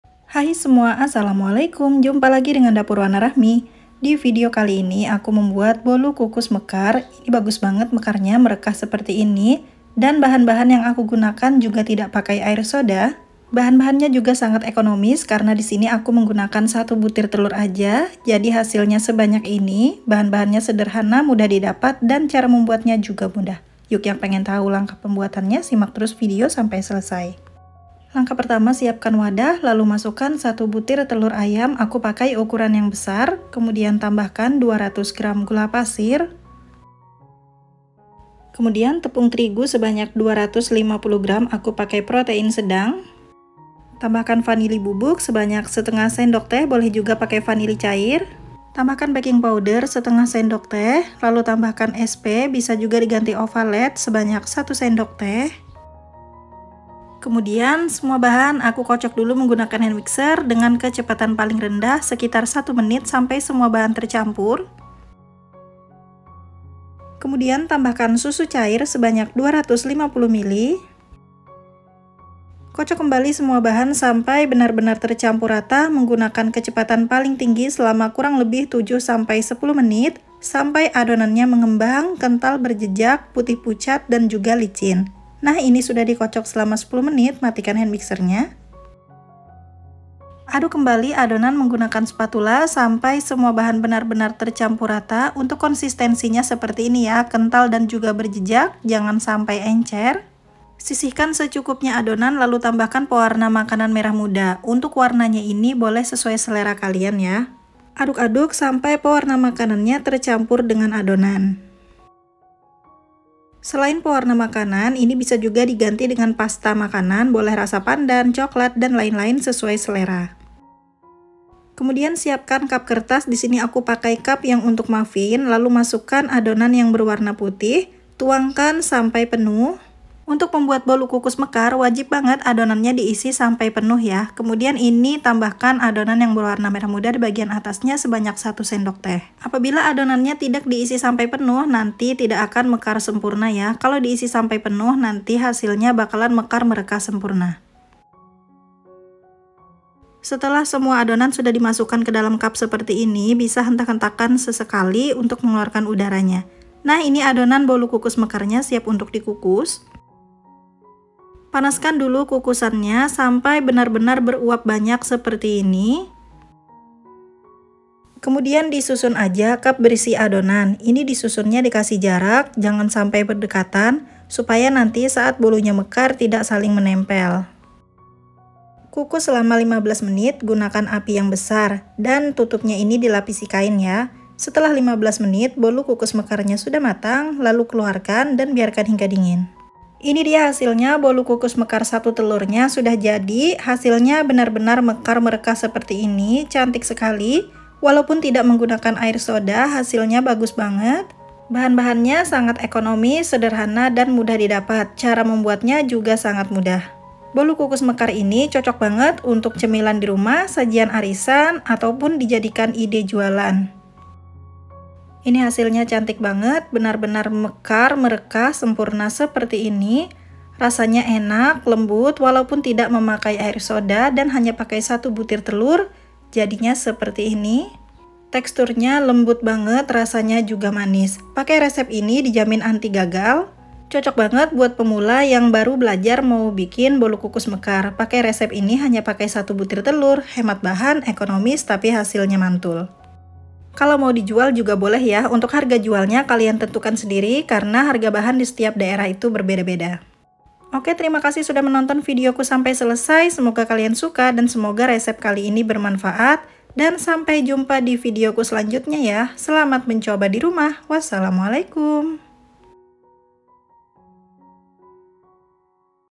MEKAR MEREKAH SEMPURNA HANYA PAKAI 1 BUTIR TELUR!! Bolu kukus mekar lembut anti gagal
Assalamualaikum semuanya kali ini aku membuat bolu kukus mekar merekah ekonomis hanya pakai 1 butir telur aja.
rasanya enak lembut dan tampilannya cantik.
bahan nya sangat sederhana dan cara membuatnya mudah, yuk langsung aja ke resepnya.

bahan:
1 butir telur
200 gr gula
250 gr tepung terigu
1 sdt SP/ovalet
1/4 sdt vanili bubuk
1/2 sdt baking powder
250 ml susu cair
pewarna merah muda

kukus 15 menit api besar, jangan dibuka selama proses pengukusan.